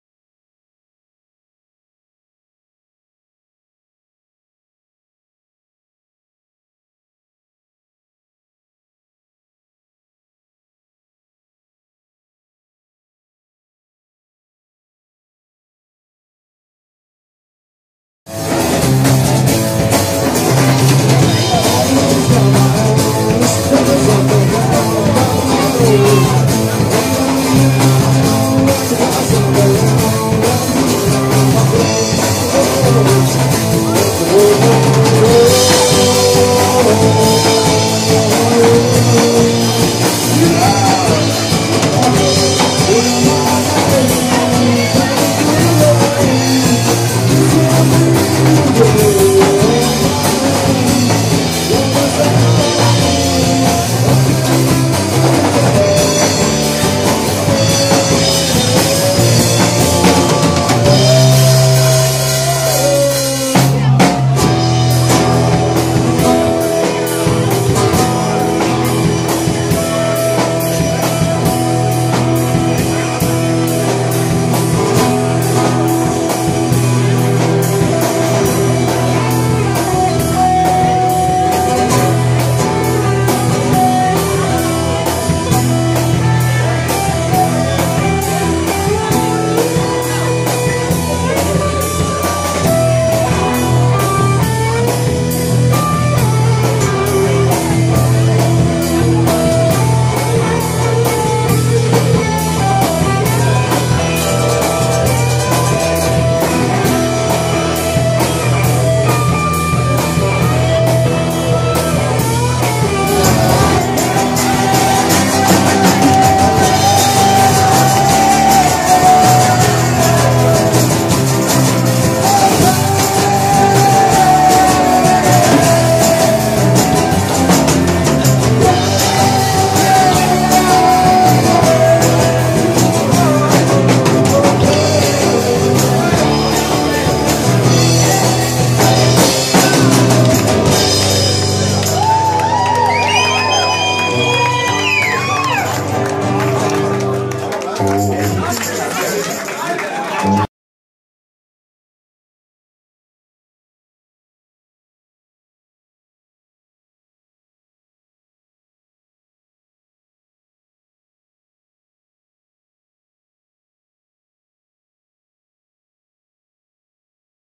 Rabula Tasa - Konzertausschnitte 2 @ Foglwuid Unterwössen 27.10.17 ® by HKH Webmaker © LosRein.de
Rabula Tasa  LIVE - Konzertausschnitte 2 @ Foglwuid Unterwössen 27.10.17  mit Tom, Gala, Petia, Gitti, Alex, Ioana, Martina, Georg, uva...
~ KONZERTE : LosRein.de/konzerte.html
~ TWITTER.com/LosRein + DIGG * * PLUS.GOOGLE.com/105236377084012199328 * losrein.BLOGSPOT.de * * calihuaba.SKYROCK.com * livecali.LIVEJOURNAL.com